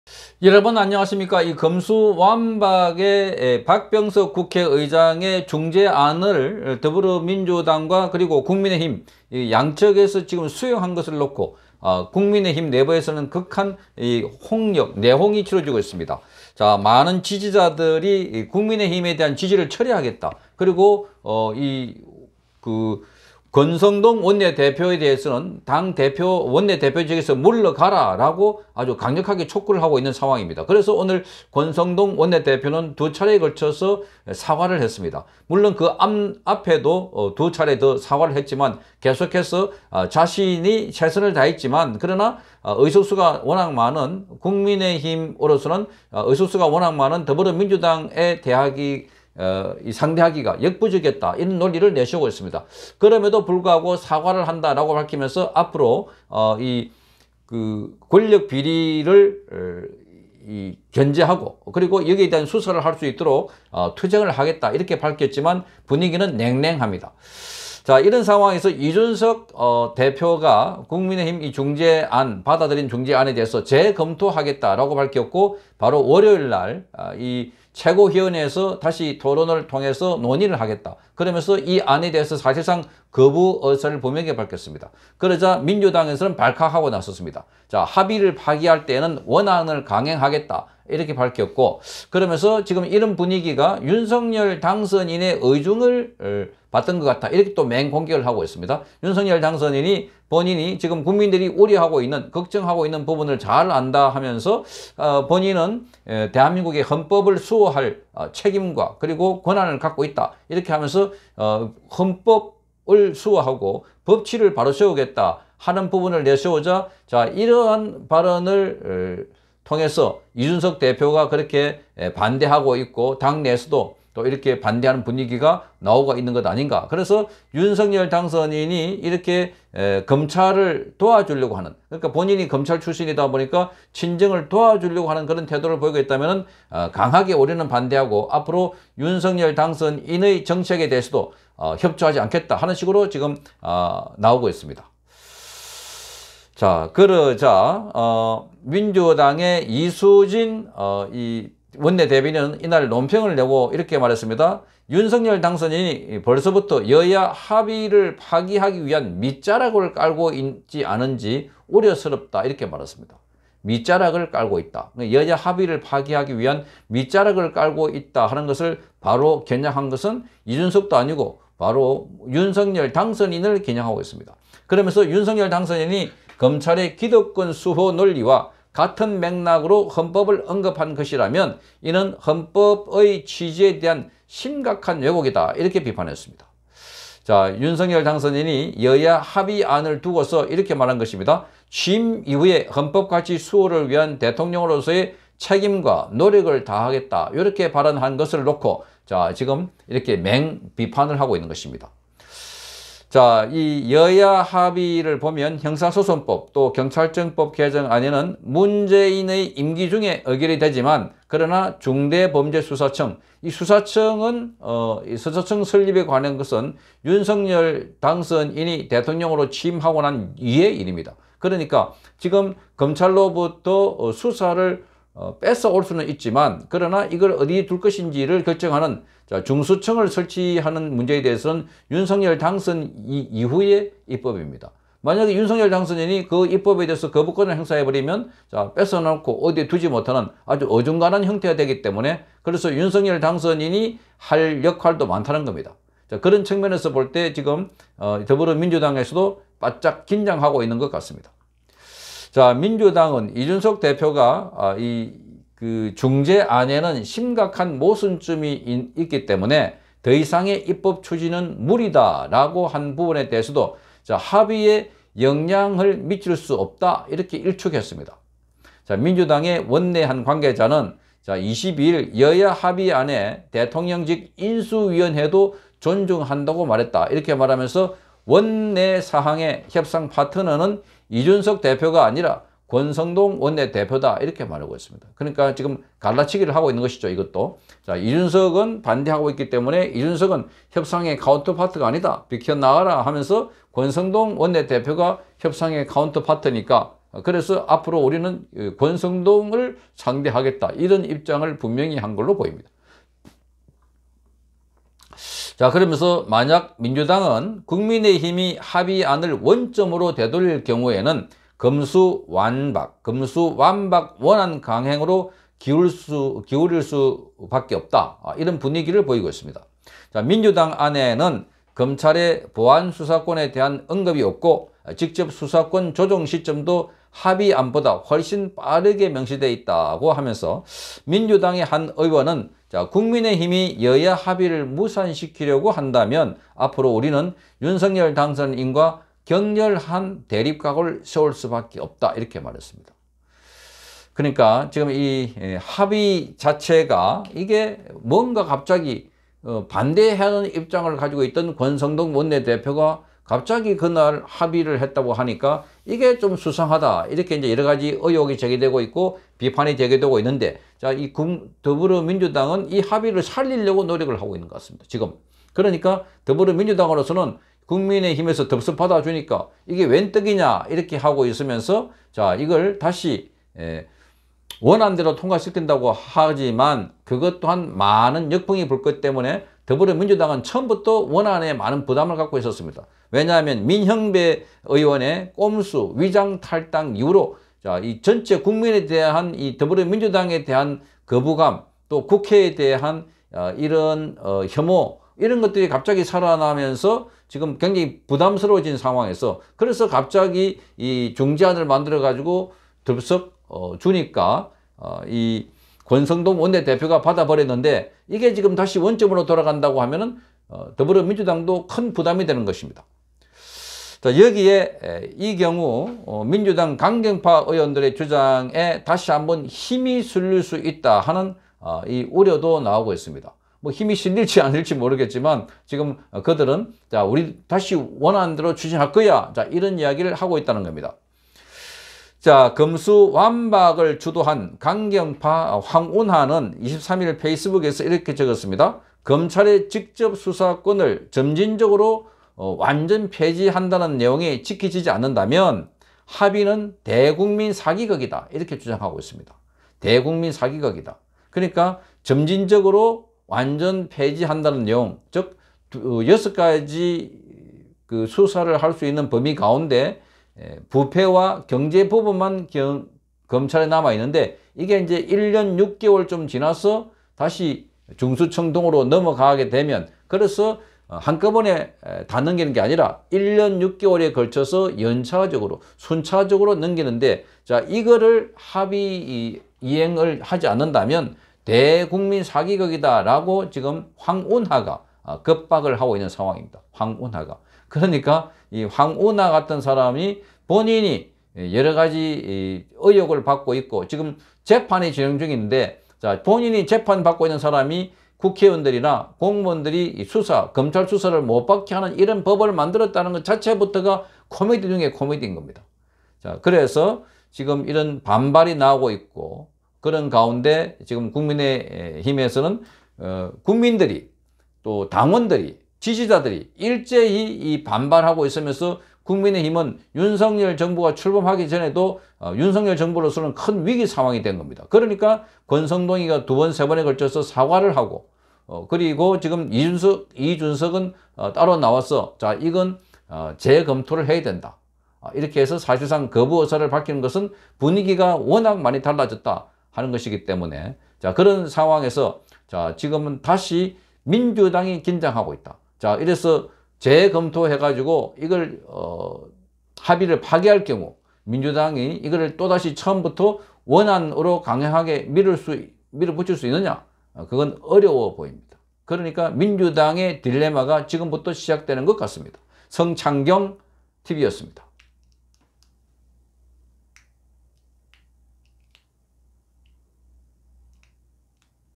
민주, 국힘이 합기안 파기할까봐 조마조마 하는 분위기. 겉으로는 겁박하지만 속으로는 불안
일방적으로 밀어 붙일 수 없어서 국힘이 파기할까봐 걱정인 민주.